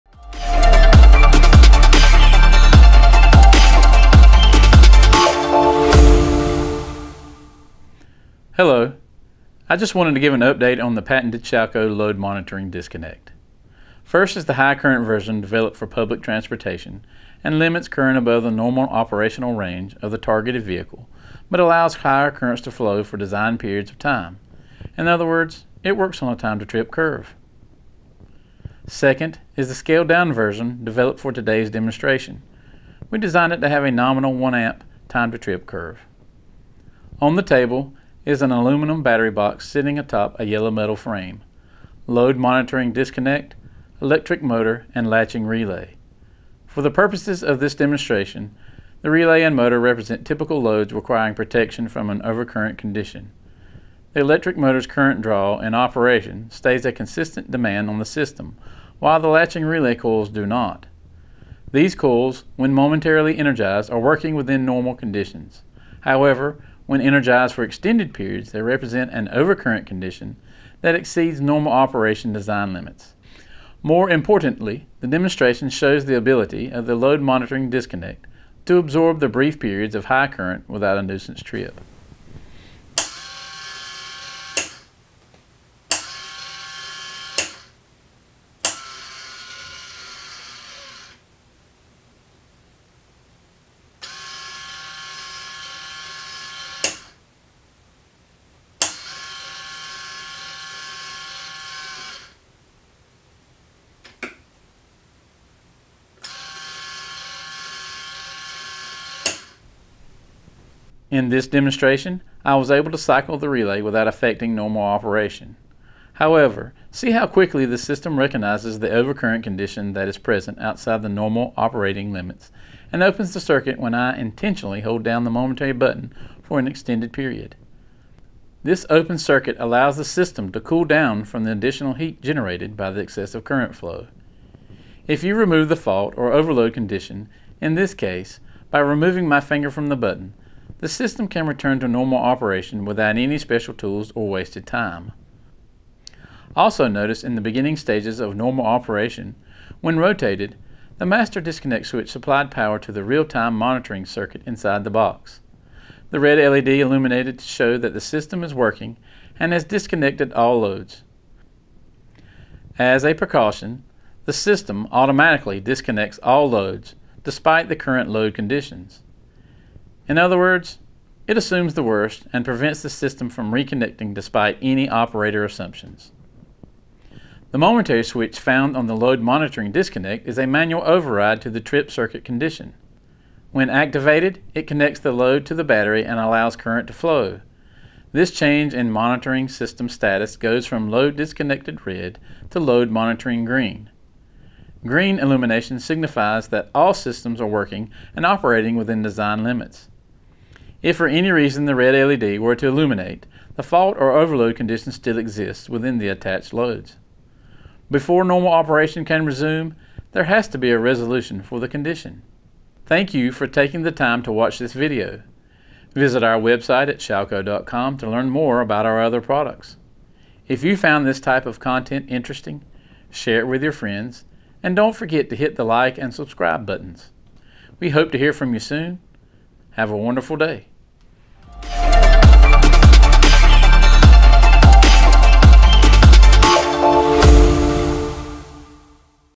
Load Monitoring Disconnect Demonstration
This video is to demonstrate the Load Monitoring Disconnect, developed by Shallco Inc. in conjunction with the Florida Department of Transportation (FDOT), and TRIPS.